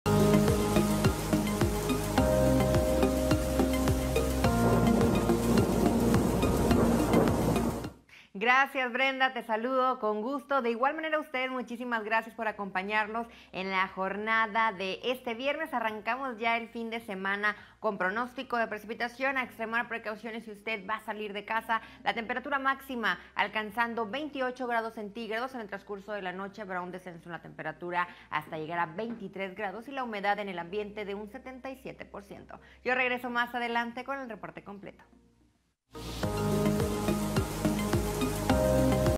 Avance clima 22 de Octubre con Yulissa Castillo
Yulissa Castillo nos mantiene bien informados con lo mas relevante del clima de Monterrey y su área metropolitana.